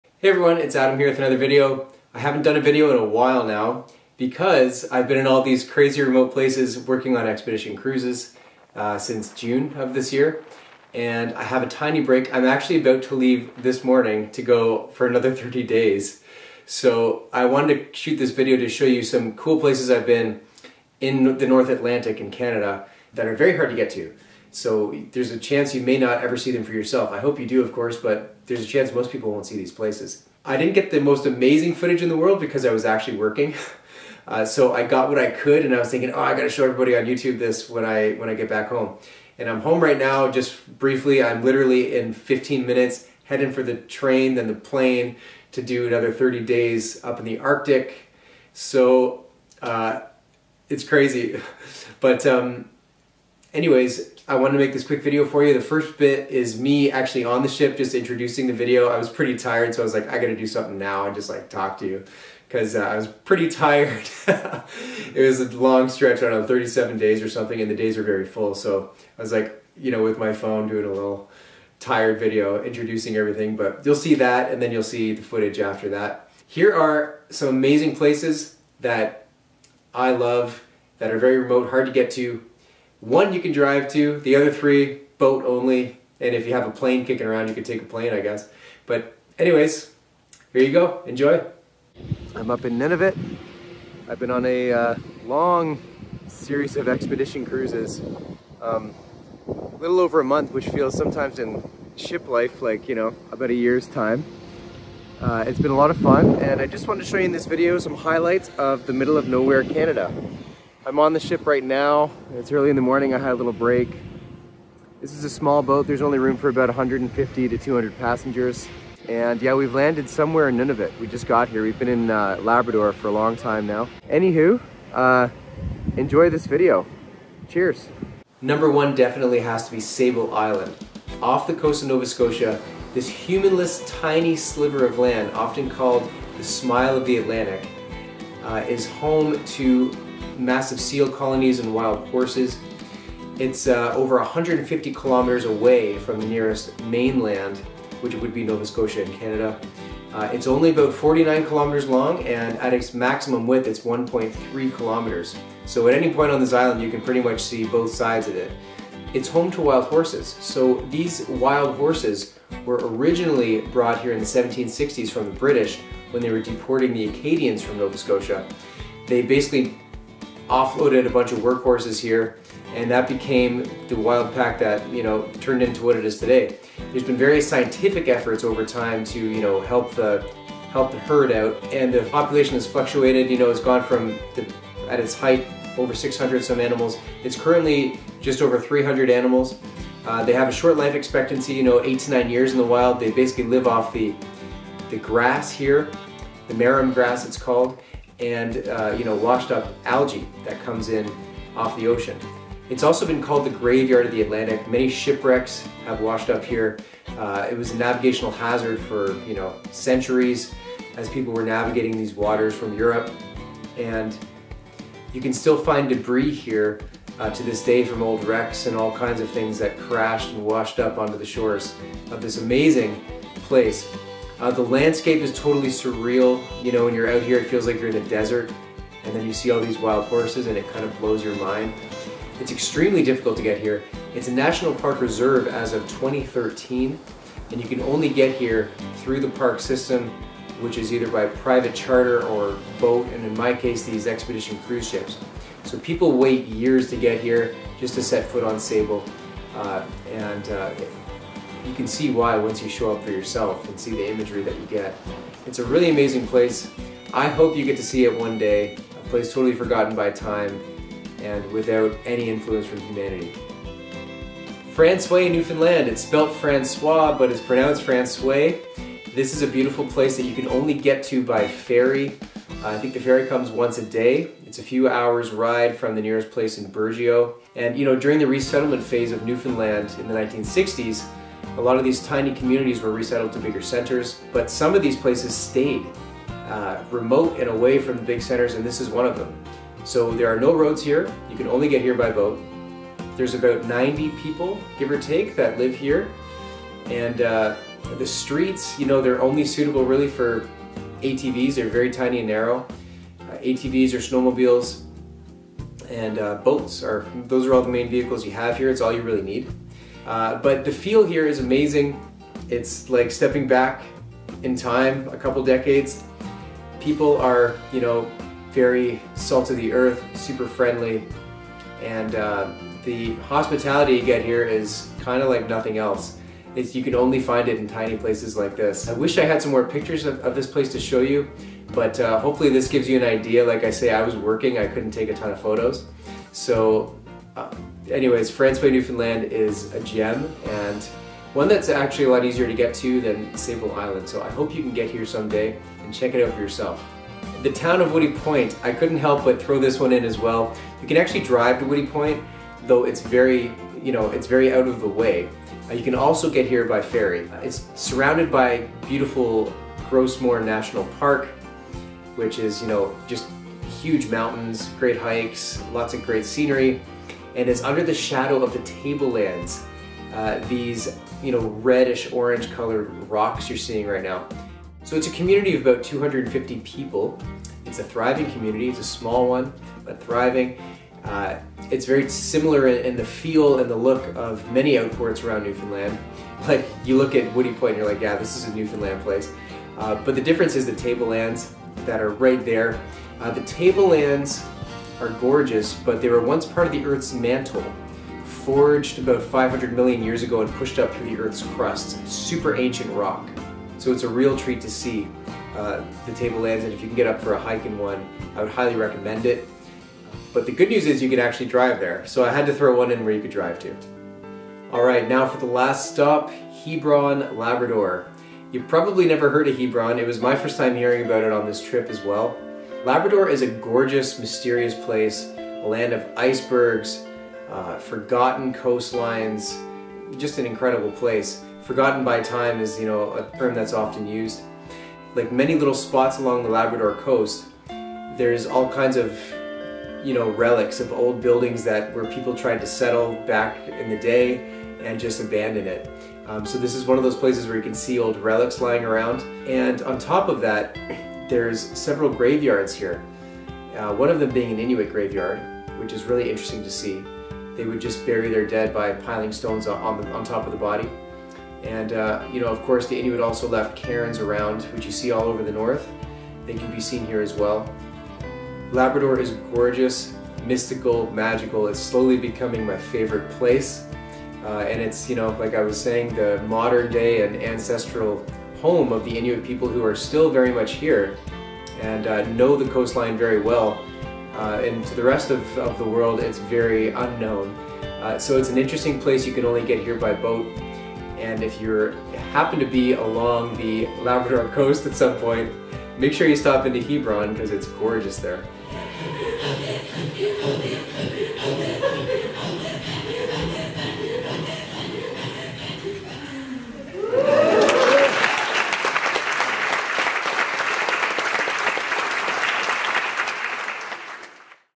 4 Remote GEMS of the North Atlantic!
Enjoy this little slice of wild Atlantic Canada! Hope you get some inspiration and ideas :)
I'm gone without reception again for a while, I'll see you all soon :)
-Adam